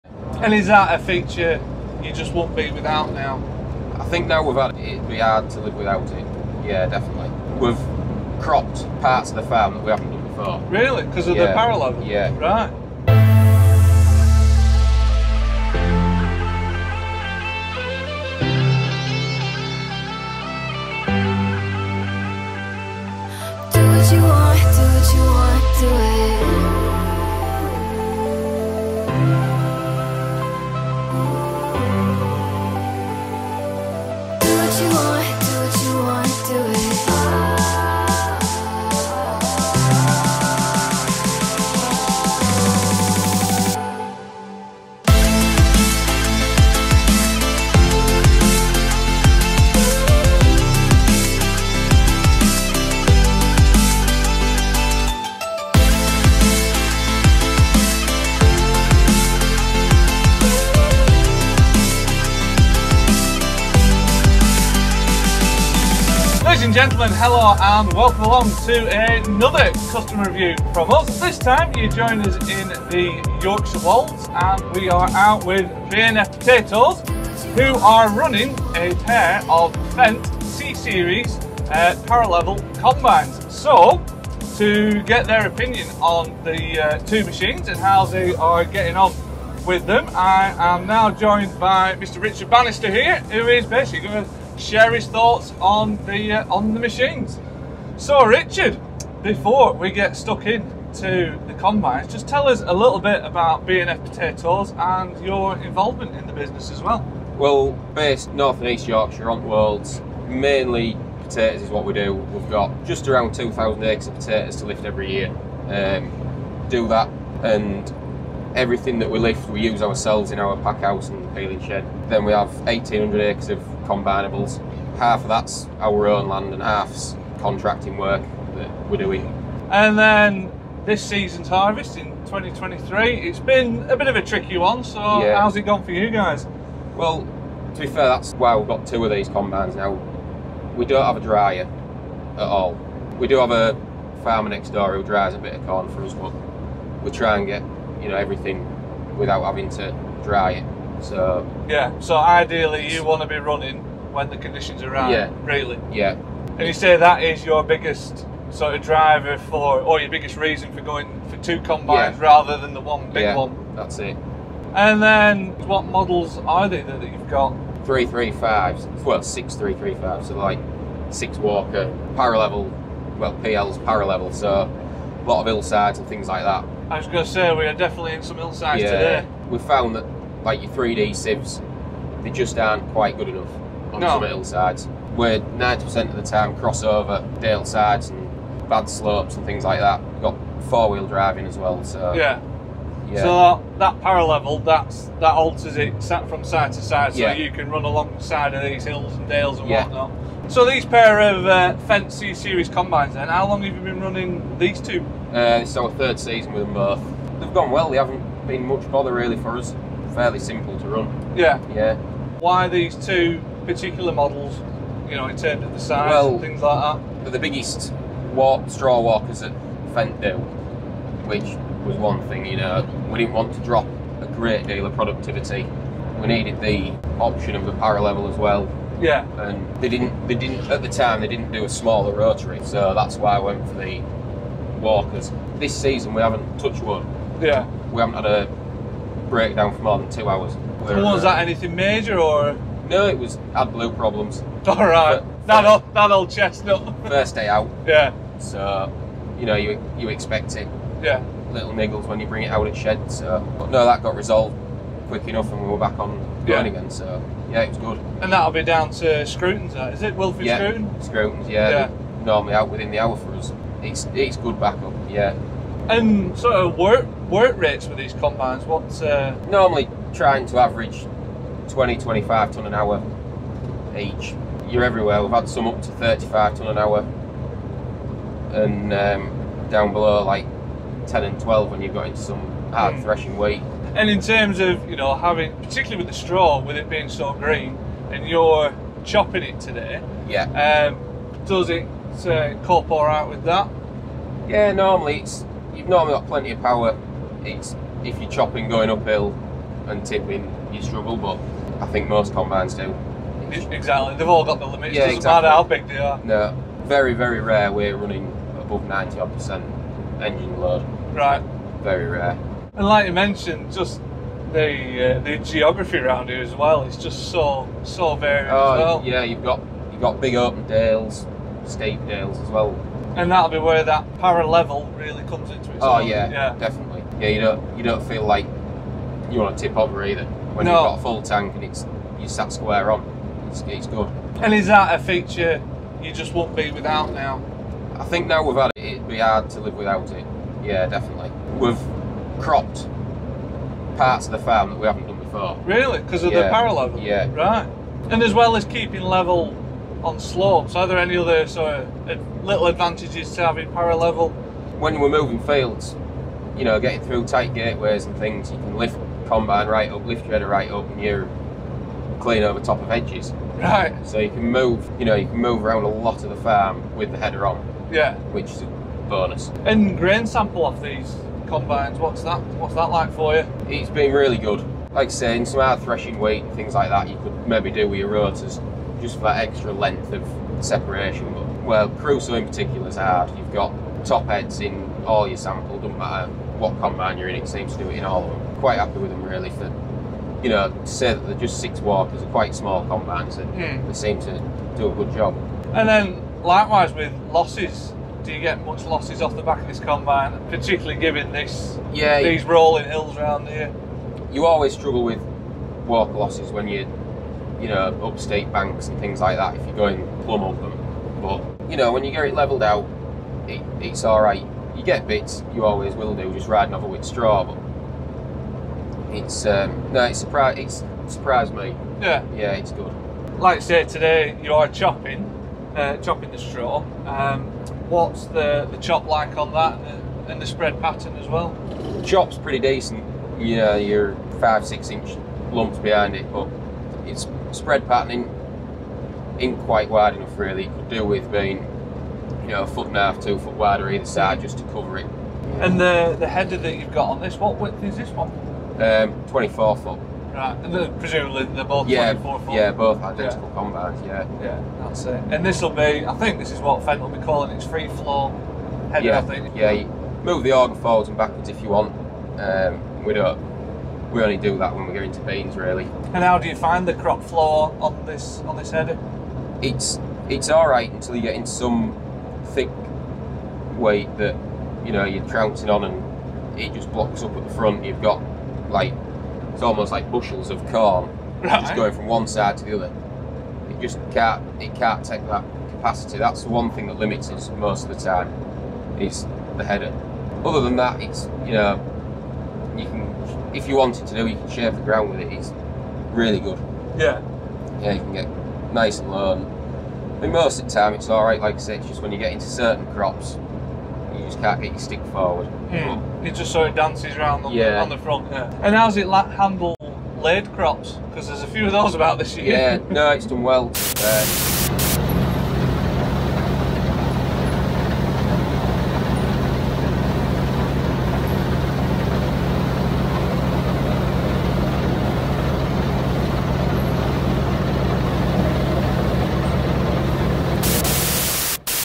Fendt 6335 C PL Six-Walker Combine: CUSTOMER REVIEW
Running a pair of Fendt six-walker, ParaLevel combines, we catch up with B&F Potatoes to find out their experiences operating the machines.

Featured machine:
Model: Fendt 6335 C PL
Engine: 8.4-litre, 6-cylinder, Agco Power
Max. power: 360hp
Transmission: 4-speed, hydrostatic
Header: 7.3m (24 feet) FreeFlow
Threshing area: 1.18sq.m
Total separation area: 9.06sq.m
Total sieve area: 5.58sq.m
Grain tank capacity: 8,600 litres
Unloading rate: 105 lit/sec
Special feature: Self-levelling Para-Level system